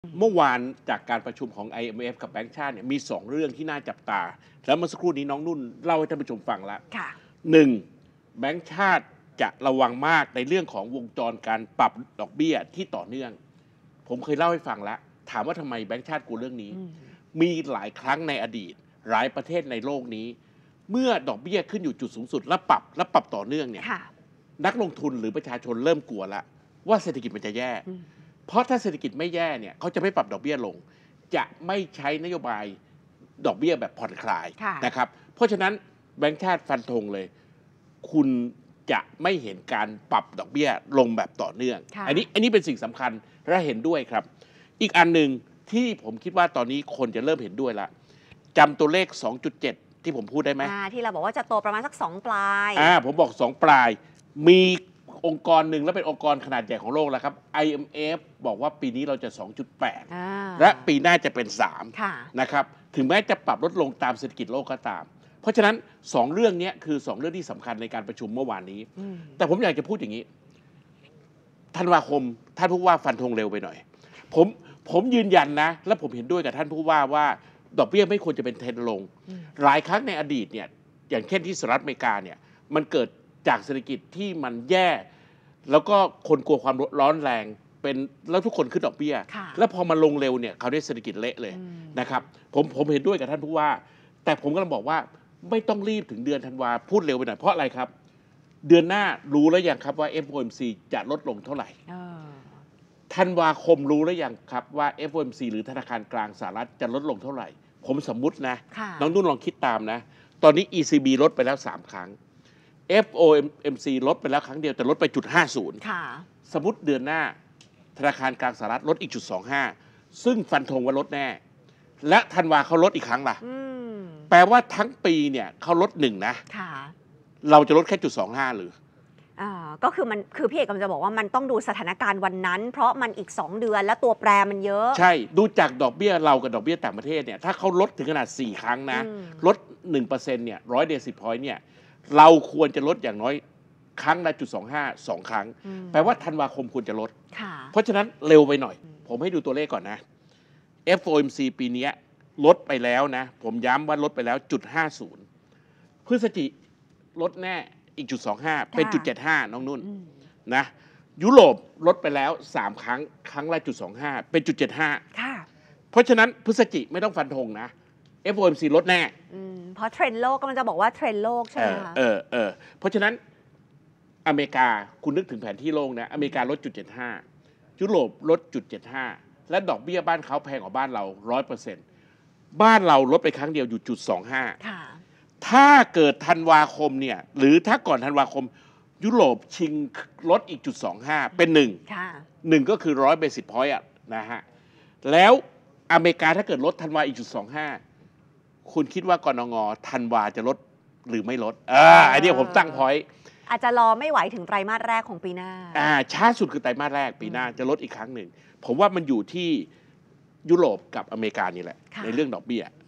สิ้นปีนี้ กนง. จะลดดอกเบี้ย? เมื่อเทรนด์โลกเขาลดแน่! | โฟกัสเศรษฐกิจ | 24 ต.ค. 67
สิ้นปีนี้ กนง. จะลดดอกเบี้ย? เมื่อเทรนด์โลกเขาลดแน่!